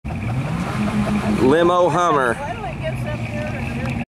Limo Hummer.
This was at my sisters prom LOL. I've never seen one of these before lol.